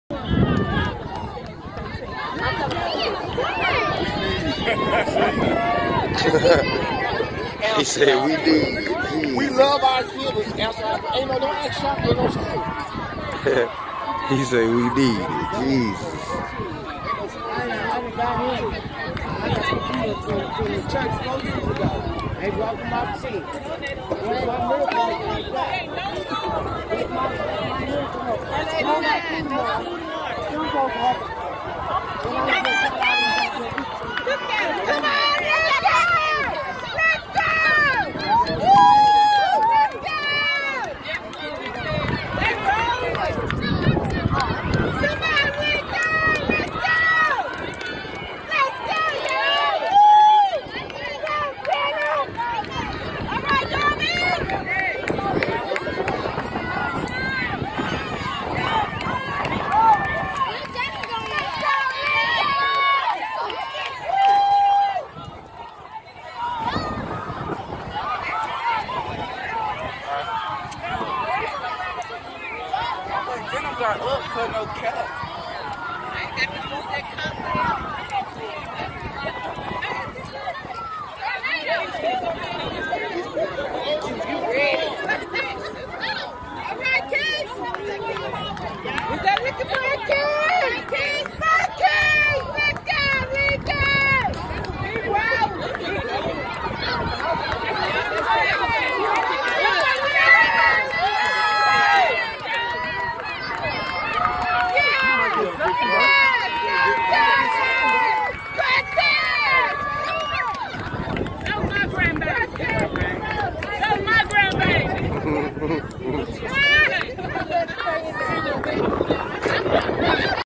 He Didn't Come To Play🏃🏾‍♂️ Middle School Boyz Track Meet She say "Thats My Grandbaby"😄😂😂
City Of Champions